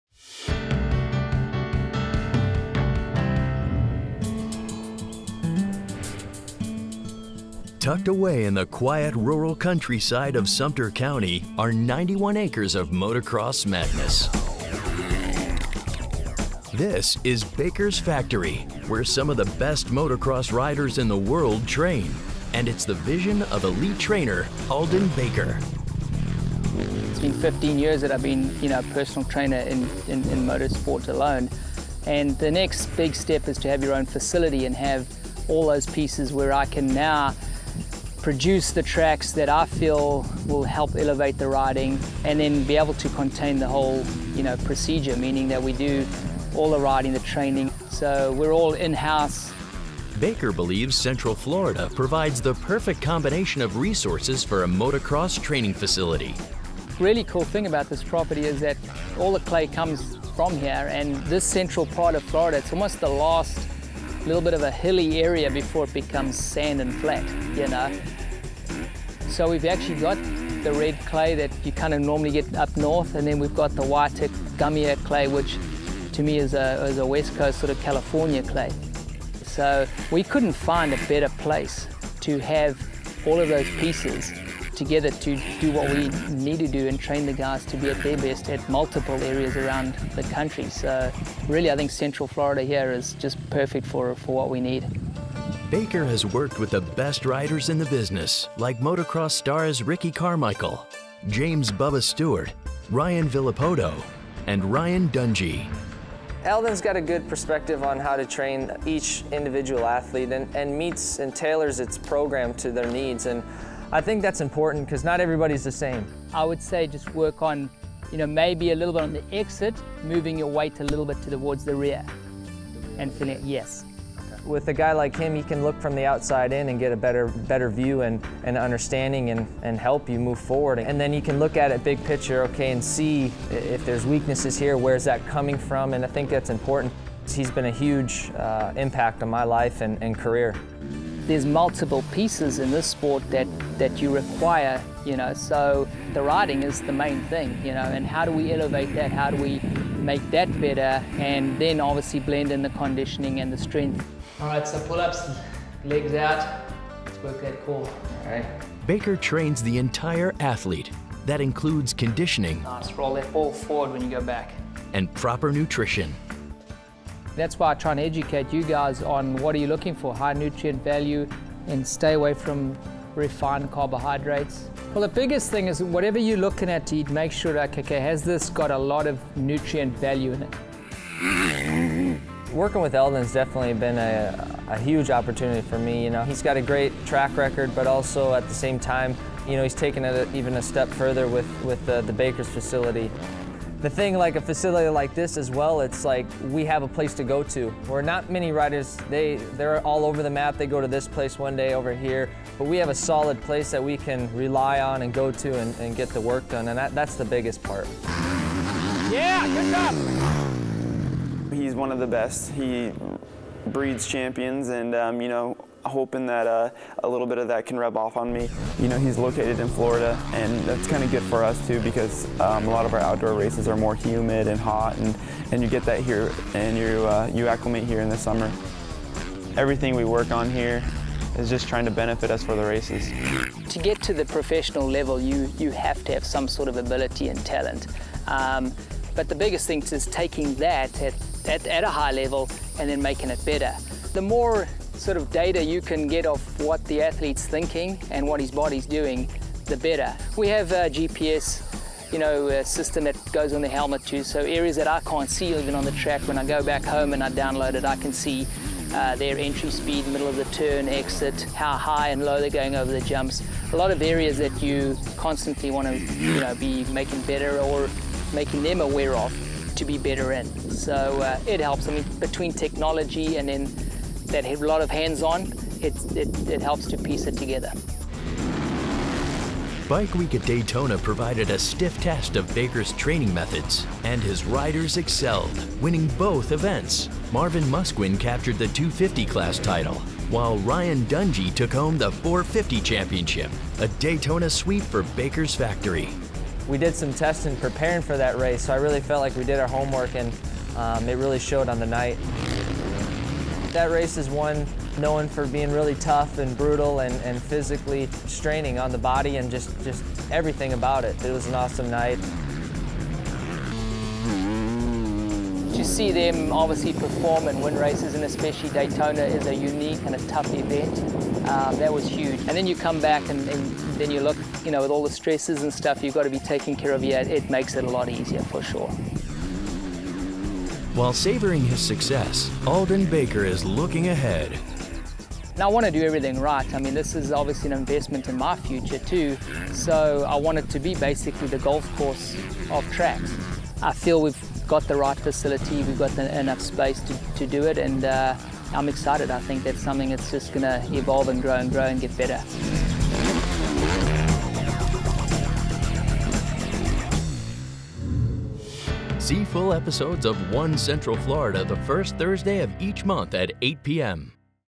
ONE Central Florida Short: Baker's Factory
In this segment of ONE Central Florida, roar over the hills of Sumter County at one of the top motocross training facilities in the country, headed by Aldon Baker, a renowned trainer from South Africa that now calls Central Florida home.